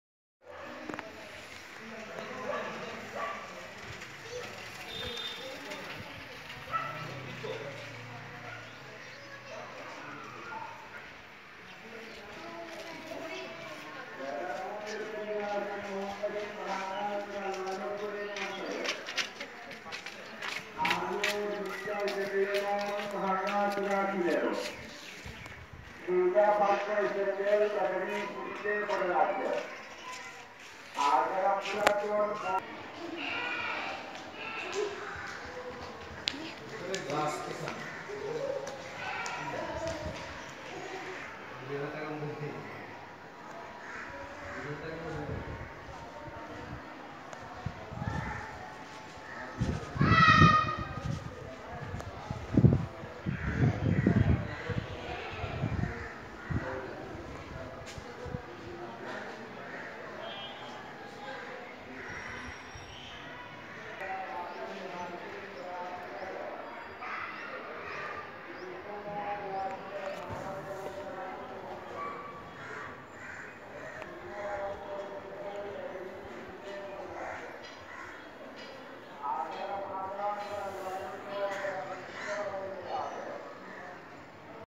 Bozratek Islamia jame Masjid, Volahat, Chapai Nawabganj, Bangladesh
Old mosque of Bangladesh. I prayed my Nafl Namaj in this mosque today.